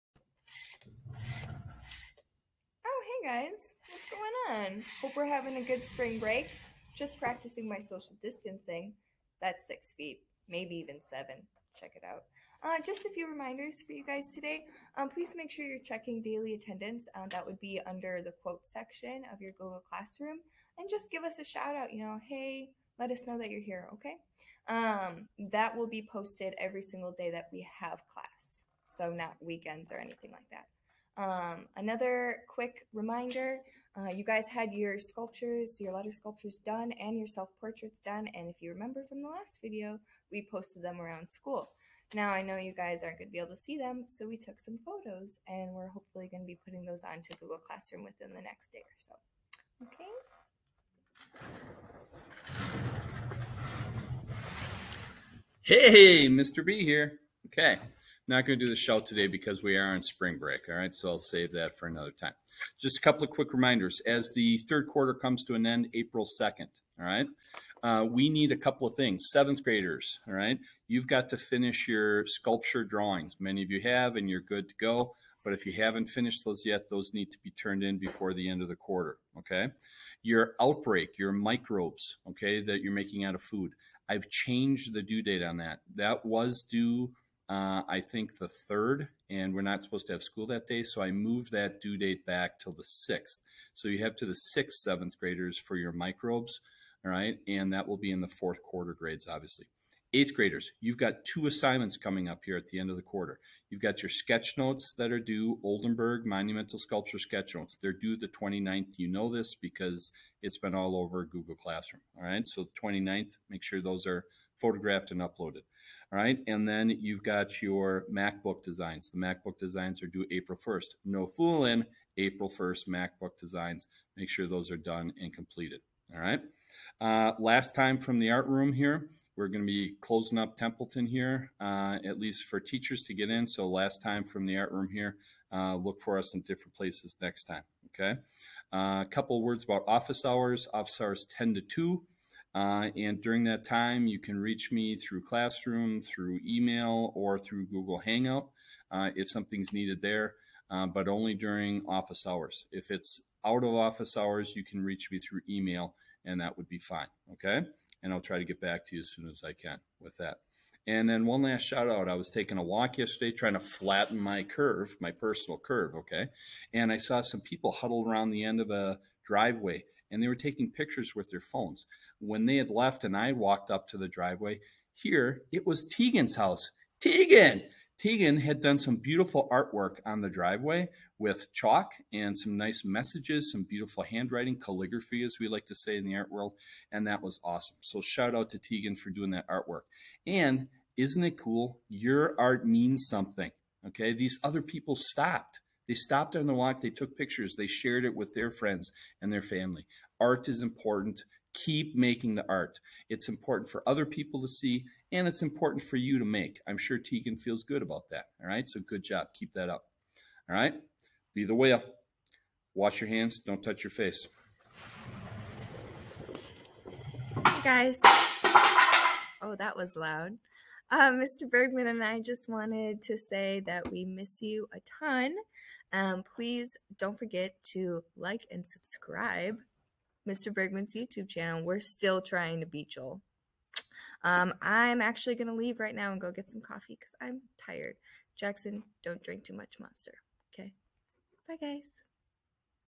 Last message from TMS Art room
reminders about end of 3rd quarter/due dates/attendance.